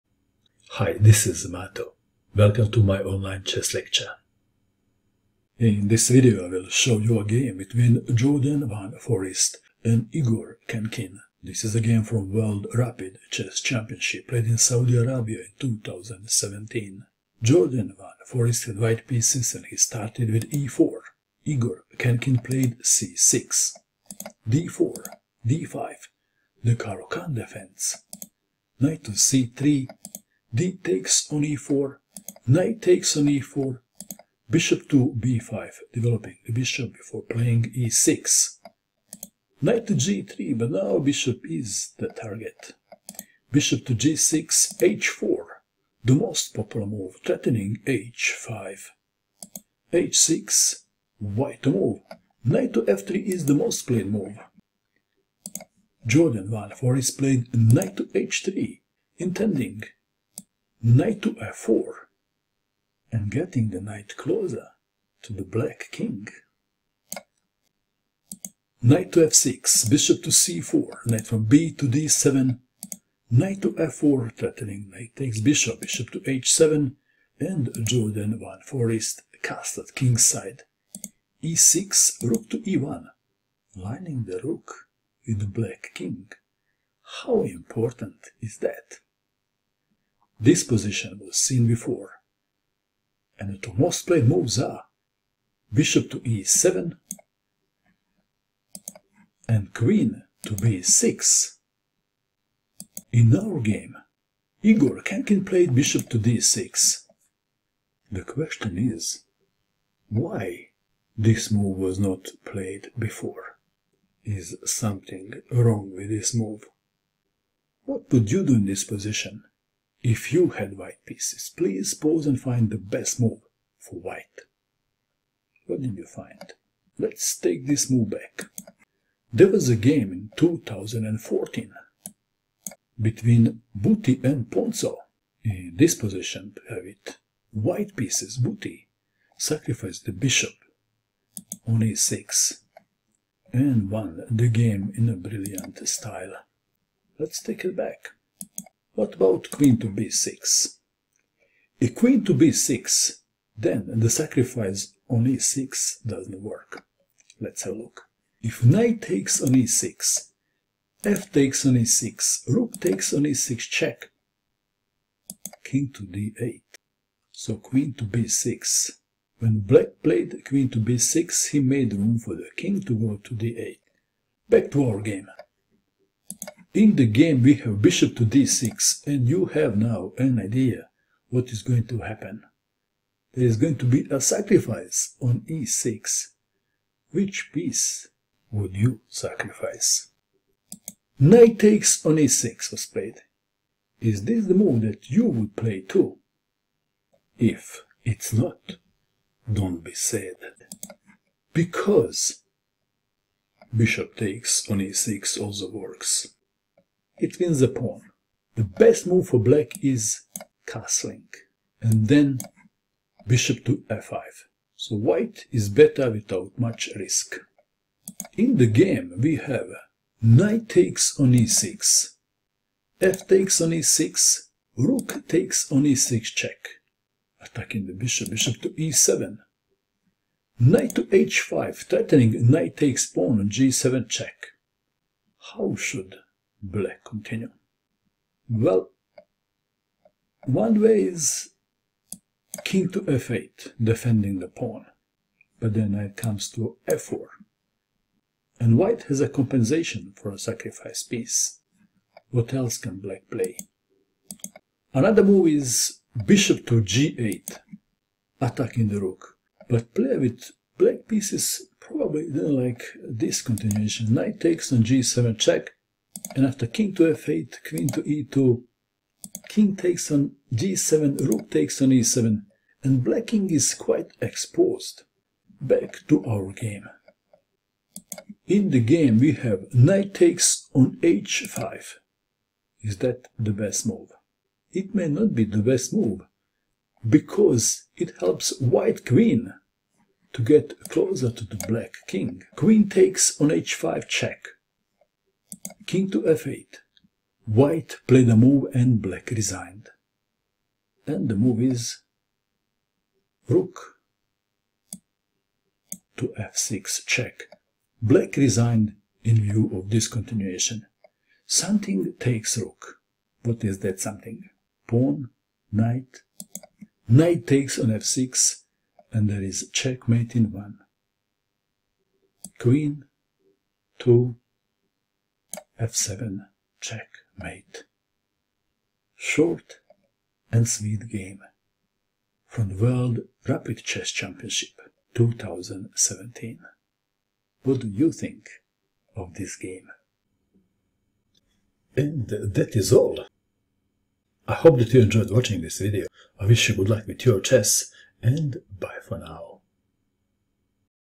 Brilliant Miniature from World Rapid Championship 2017
A Gem From Riyadh 2017
Jorden van Foreest vs Igor Khenkin
World Rapid Championship 2017, Riyadh KSA, rd 15, Dec-28
Caro-Kann Defense: Classical Variation. Main lines (B18)
1. e4 c6 2. d4 d5 3. Nc3 de4 4. Ne4 Bf5 5. Ng3 Bg6 6. h4 h6 7. Nh3 Nf6 8. Bc4 Nbd7 9. Nf4 Bh7 10. O-O e6 11. Re1 Bd6 12. Ne6 fe6 13. Re6 Be7 14. Nh5 Nh5 15. Qh5 Kf8 16. Rf6
 1-0